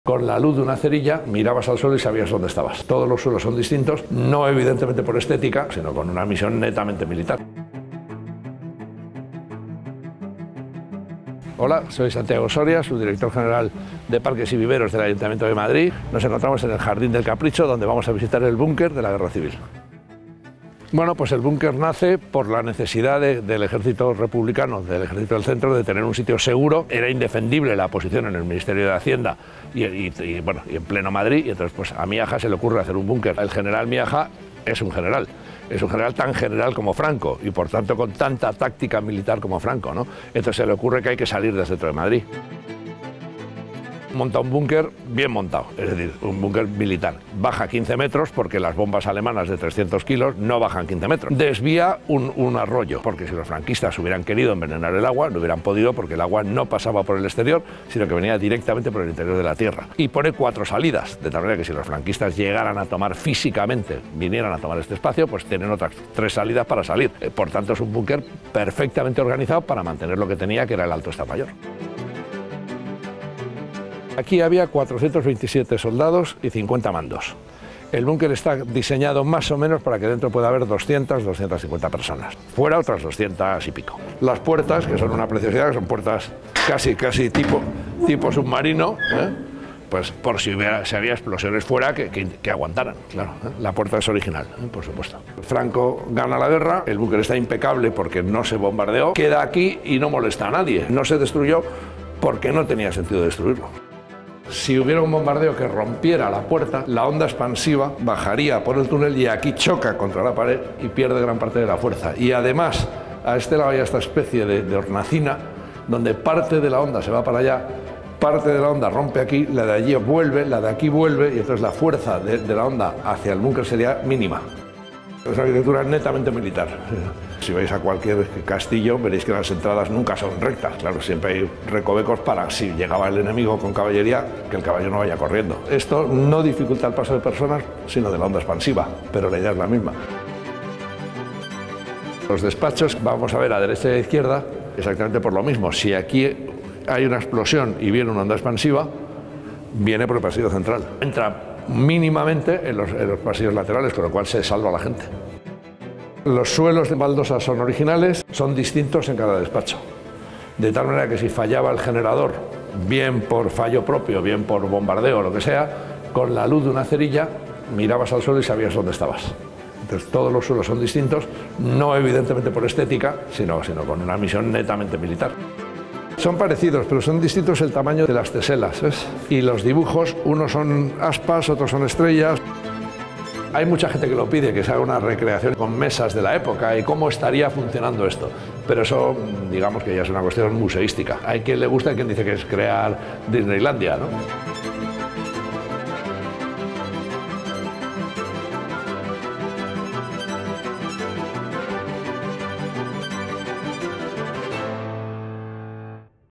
Lugares secretos de Madrid: el búnker del Capricho | Madrid Secreto
El búnker del Capricho –o búnker de la Posición Jaca (nombre en clave que recibía el Cuartel General del Ejército Republicano del Centro)– es una de las construcciones militares más importantes de Europa por el buen estado en el que se encuentra. Se trata de un espacio construido a 15 metros bajo tierra y que cuenta con unos 2.000 metros cuadrados.

Lo hemos visitado de la mano de Santiago Soria, subdirector general de parques y viveros del Ayuntamiento de Madrid, que nos ha descubierto alguno de los secretos de un espacio único que puedes visitar a través de los programas Pasea Madrid y Madrid Otra Mirada.